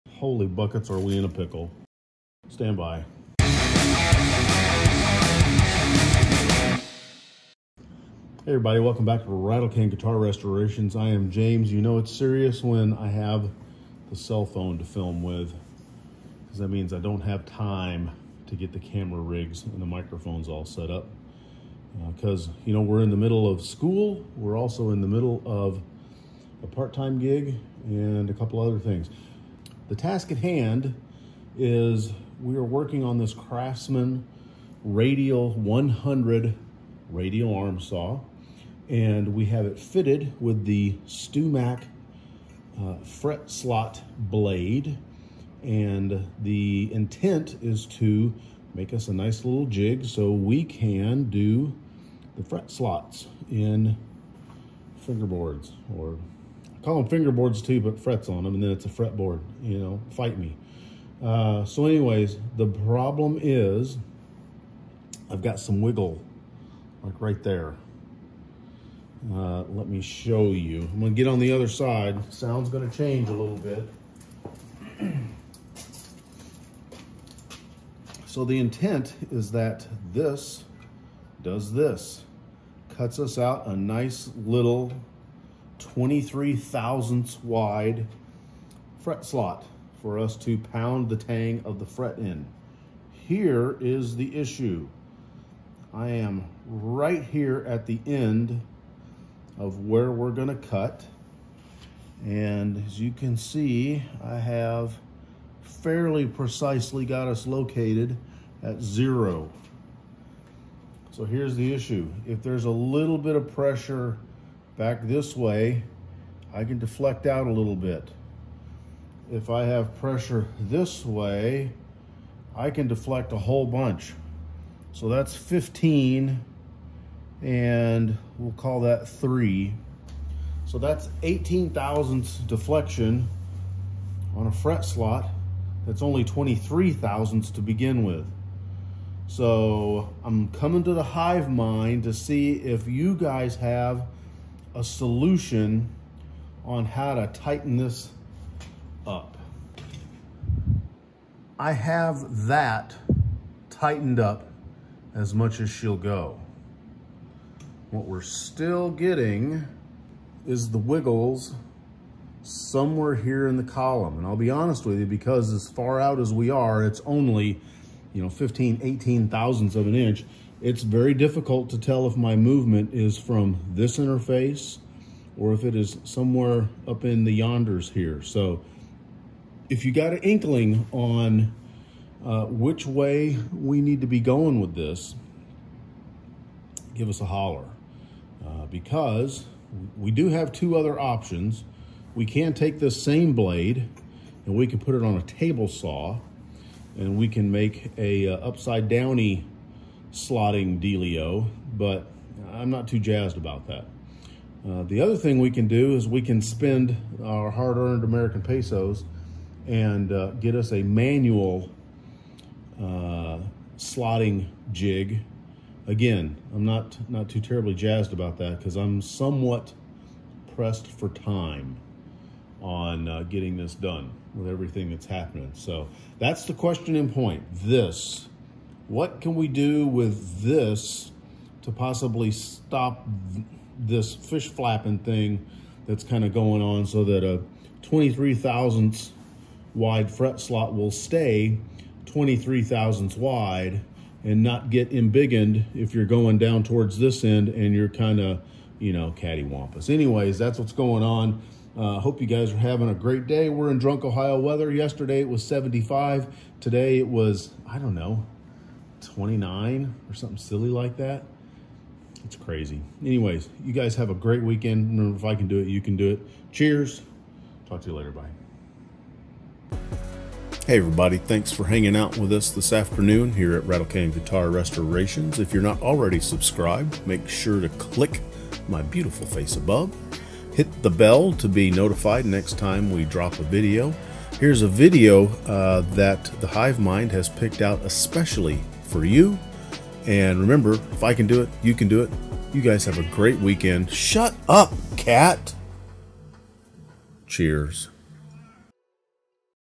Radial Arm Saw has the wiggles! | RATTLECAN GUITAR RESTORATIONS by James O’Rear
I have a 1960s era Craftsman radial arm saw RAS that I want to convert into a dedicated fret slotting saw. The e I have is that the arm, even when securely locked into the 90-degree position, has a bit of wiggle to it. About .018" which is a massive amount given the slot is only .023" wide.

Any ideas where to look to remove the play? Tighten something? Shim something? Weld something? Let me know what ideas you have.